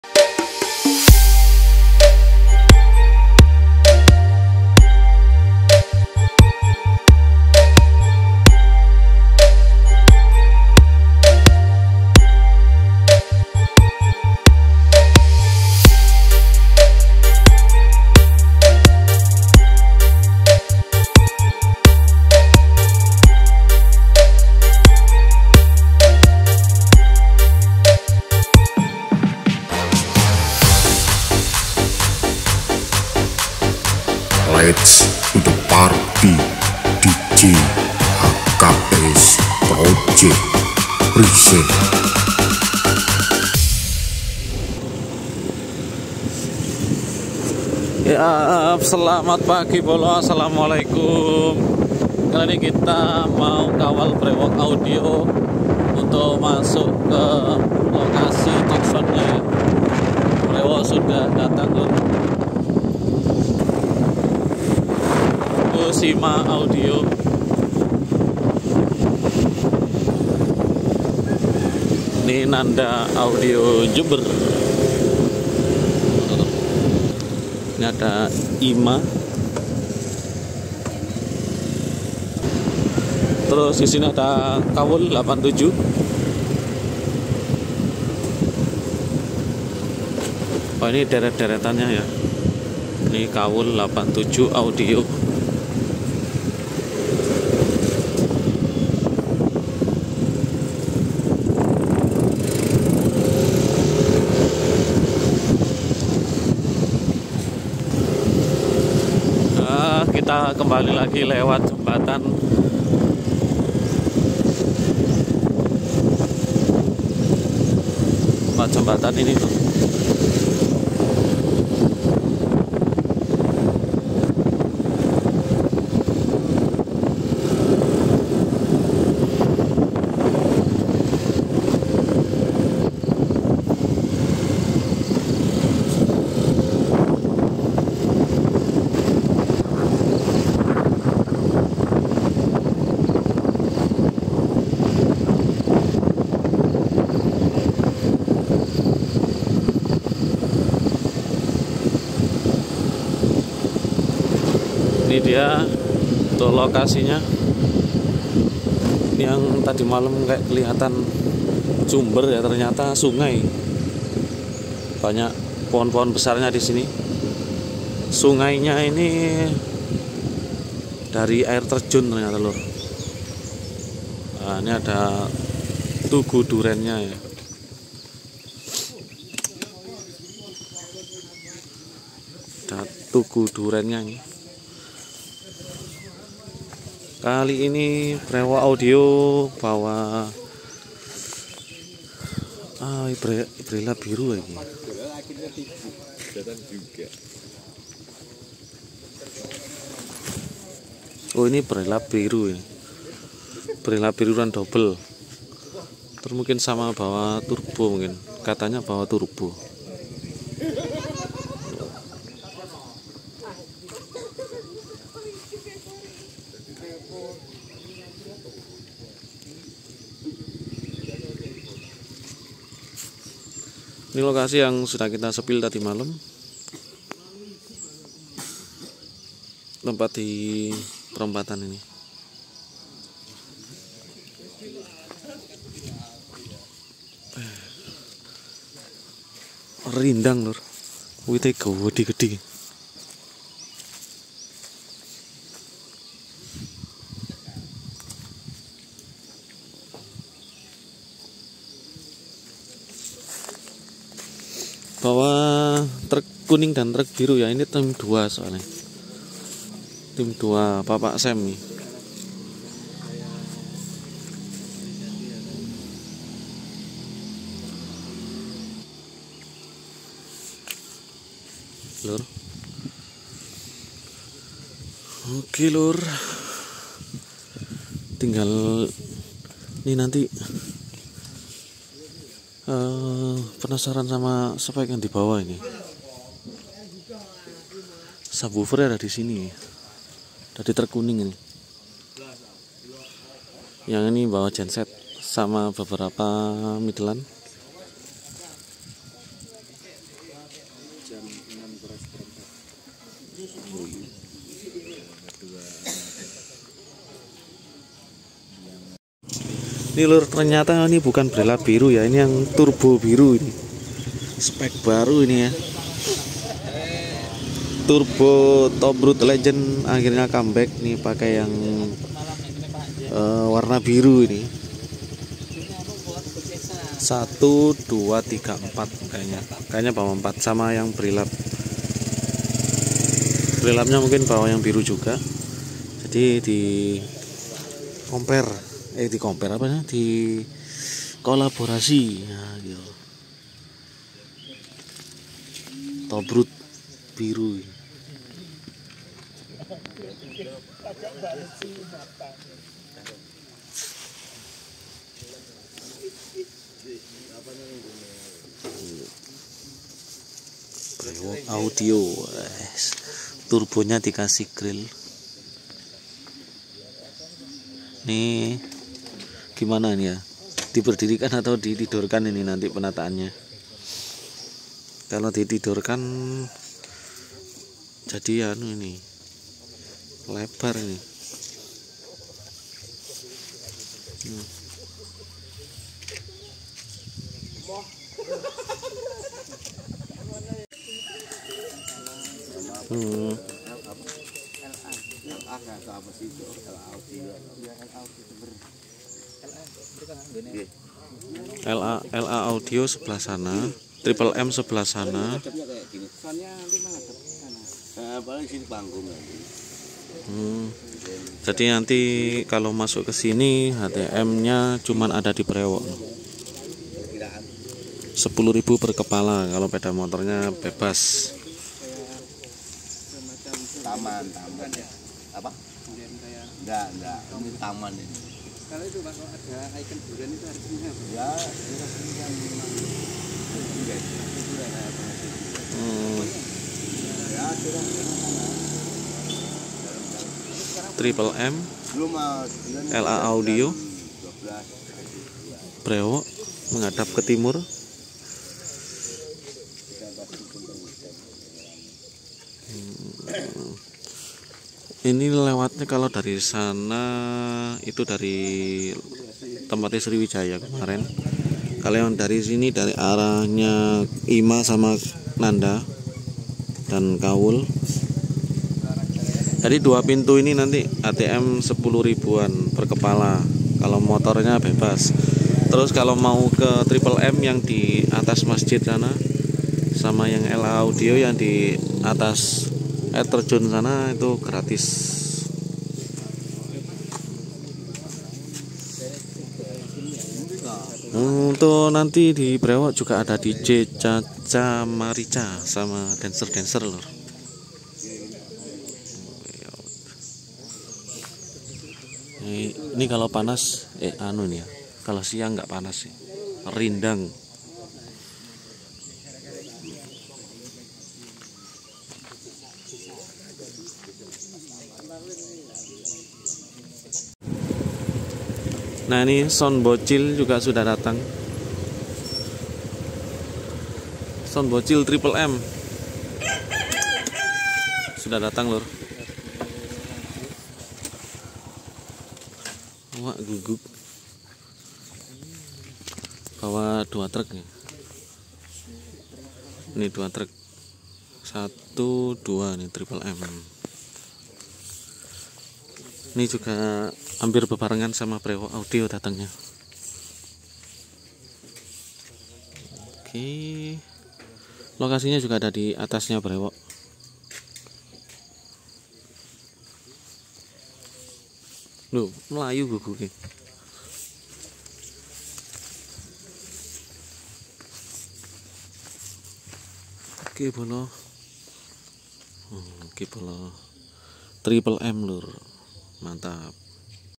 Maskotnya mendarat, BREWOG bawa box Tobrut 🔥BREWOG & TRIPLE M akhirnya menginjakkan Kaki di BLINGOH
KARNAVAL BLINGOH JEPARA sudah mulai bergeliat loading para sound system Jawa Timur dan sound system Tuan Rumah..
sound" Jawa Timur
BREWOG
NANDA
IMA JEMBER
SRIWIJAYA
AR JEMBER
KAWOL 87
GRAND MUSIC
SPL AUDIO
LA AUDIO JEMBER
TRIPLE M
SIMA AUDIO tuan Rumah

cek sound dibagi 2hari ,,tgl 21 SRIWIJAYA, KAWOL 87 & GRAND MUSIC...
tgl 22 sound yg lain ...
tgl 23 karnaval start jam 1 siang